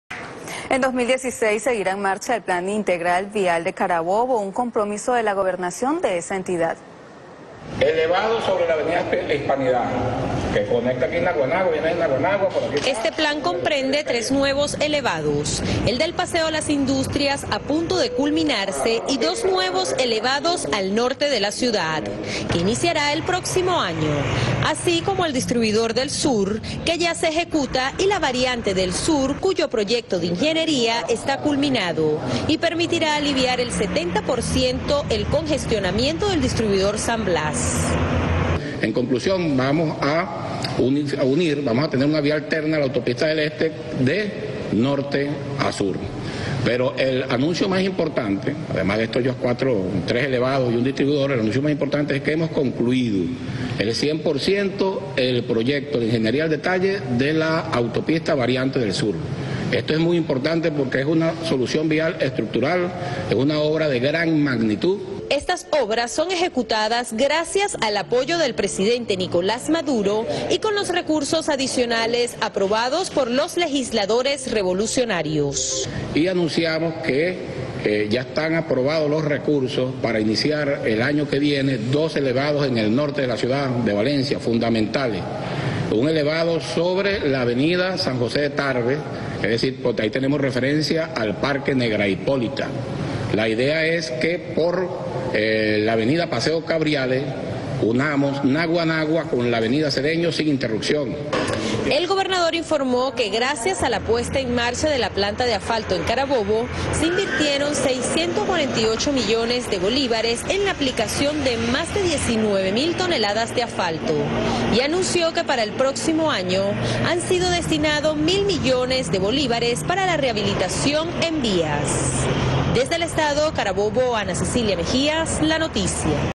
Ameliach: Culminamos proyecto de Ingeniería de la Variante del Sur
El gobernador del Estado Carabobo, Francisco Ameliach anunció que fue concluido totalmente el Proyecto de Ingeniería en detalle de la Autopista del Sur, importante obra incluida en el Plan Integral de vialidad que adelanta el ejecutivo regional, gracias al apoyo del gobierno nacional y de la mayoría chavista en la AN.